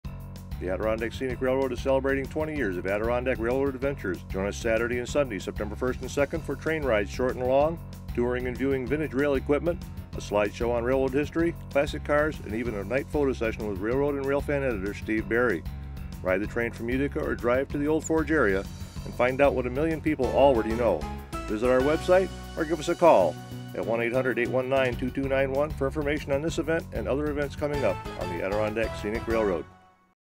20 Anniv celebration YouTube Movie
Join the Adirondack Scenic Railroad September 1st and 2nd in Thendara (Old Forge) as we celebrate 20 years of Adirondack Rail Adventures.